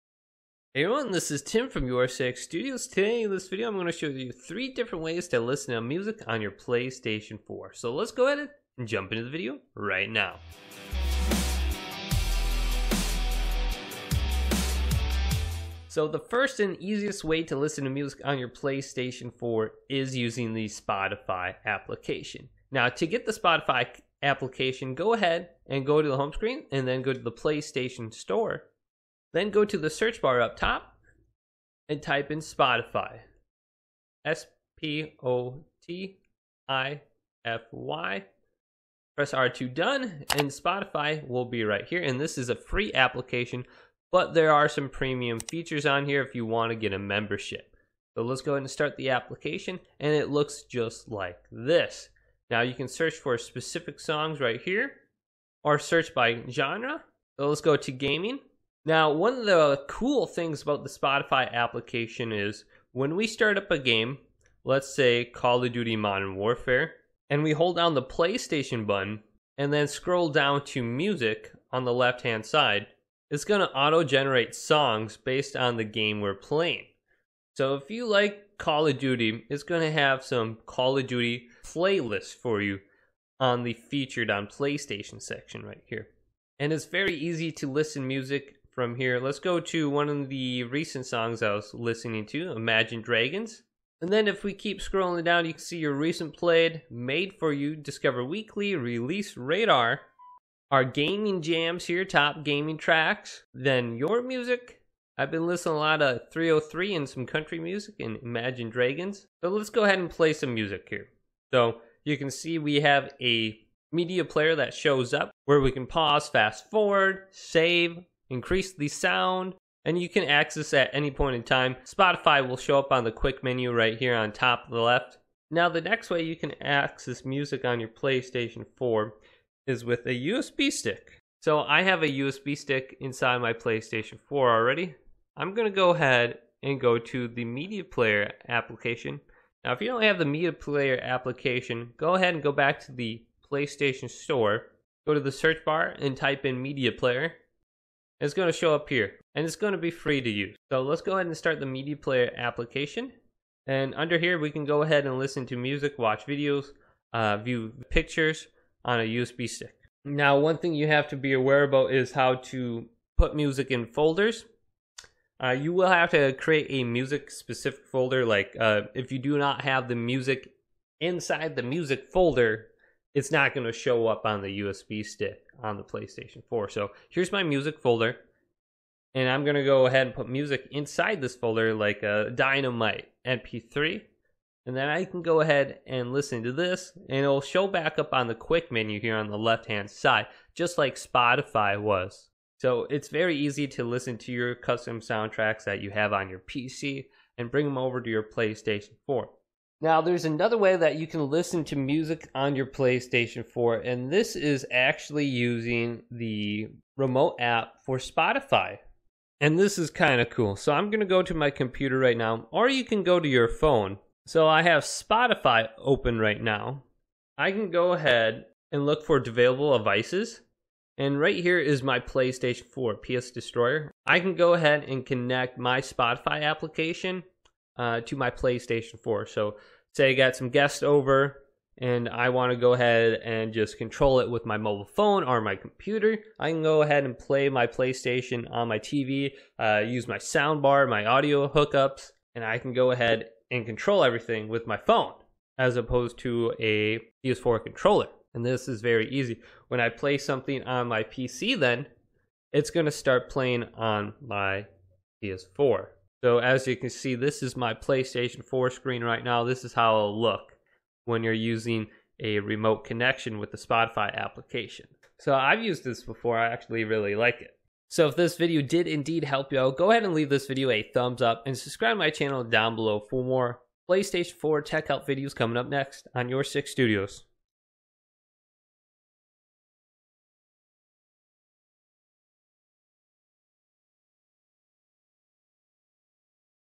3 Ways to Listen to Music on the PS4 while playing games! (Easy Method)
Do you want to know how to listen to music on the ps4 while playing games or trying to entertain guest? The PlayStation 4 has many ways to enjoy listening to music, using youtube, USB, or the spotify application. The coolest thing is being able to listen to music while playing games by pressing the ps button down and going to the music section in the quick menu. Under here, you can skip sounds, increase the volume or save a song.